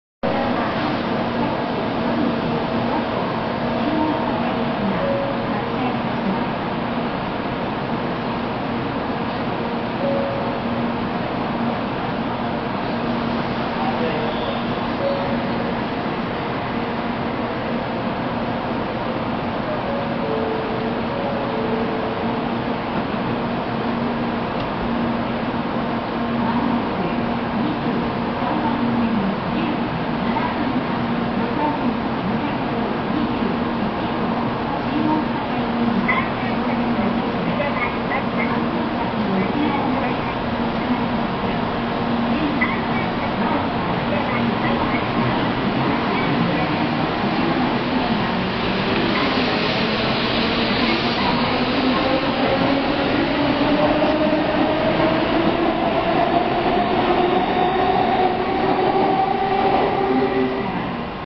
東海道新幹線品川駅 こだま号発車 安全柵（ホームドア）が閉まる
東海道新幹線品川駅のこだま号発車と安全柵（ホームドア）が閉まるところです。品川駅では列車が動き出してから安全柵が閉まります。